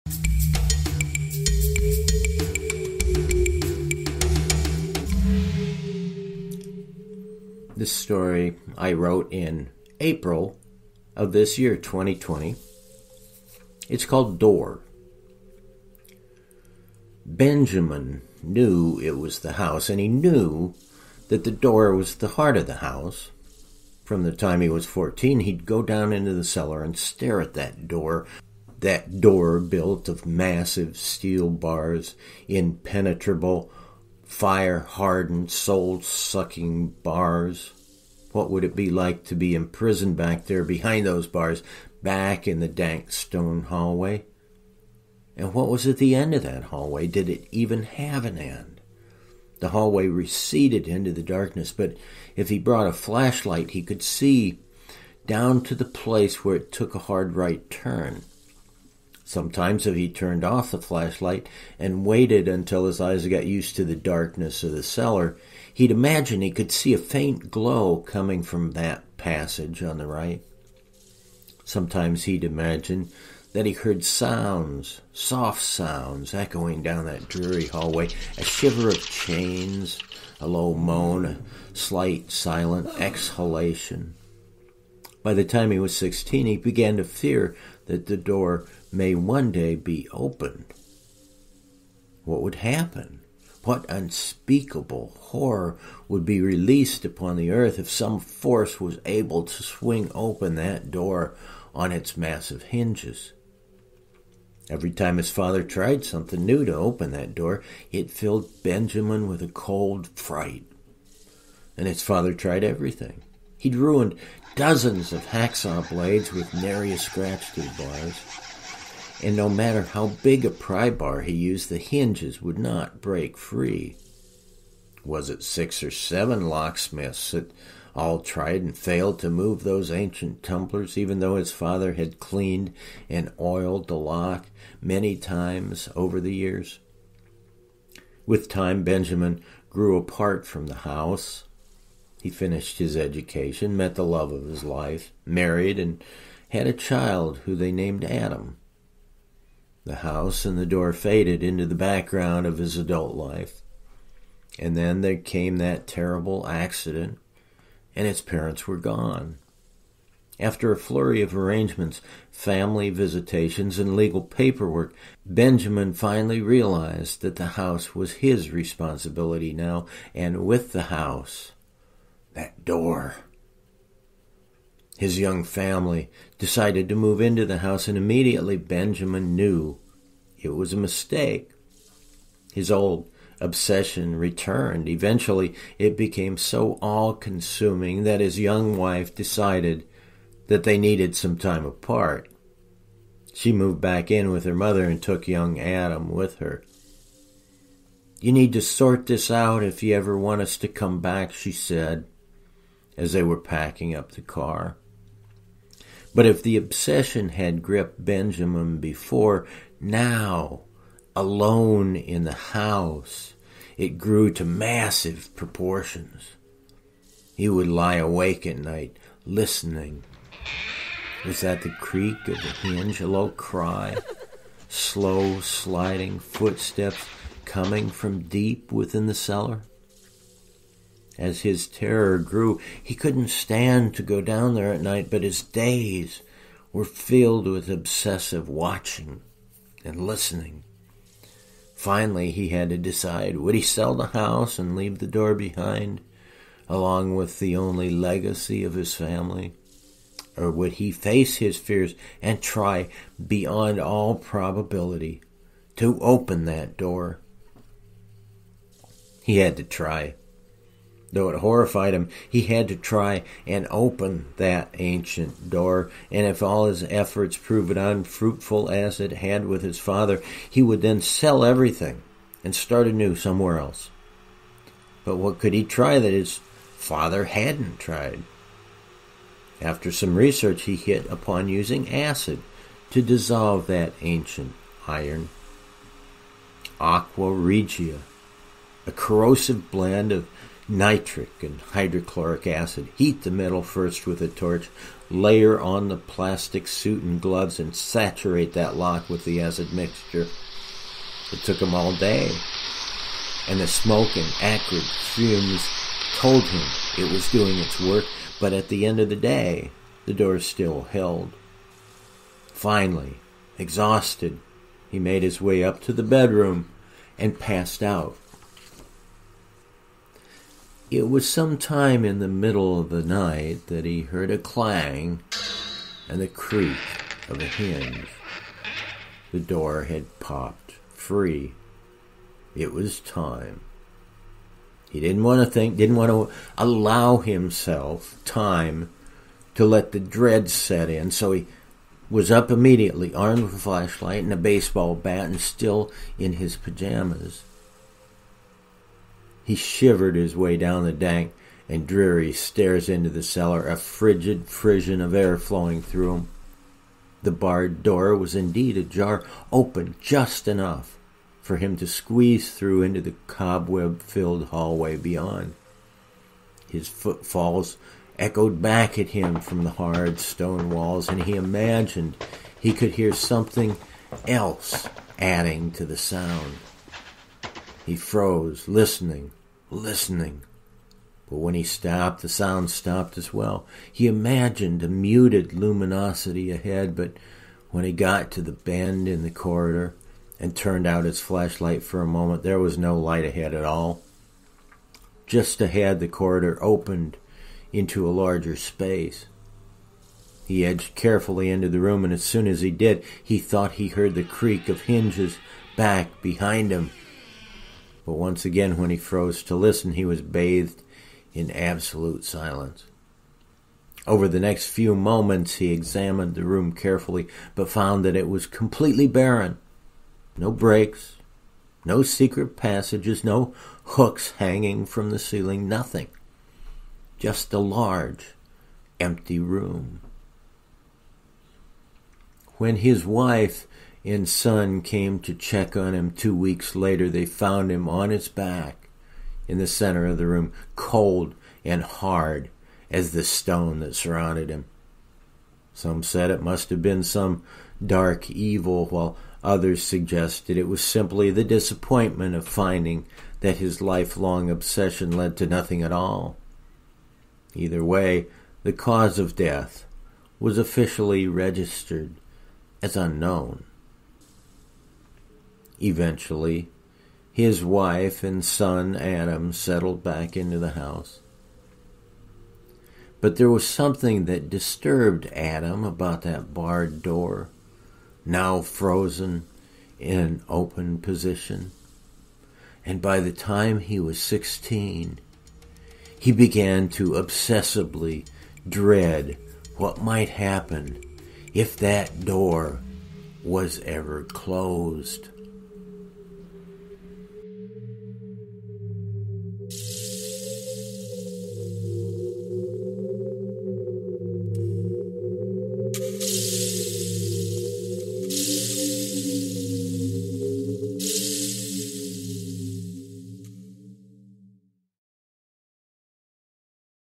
Door
A dark obsession about an ancient door is inherited, father to son.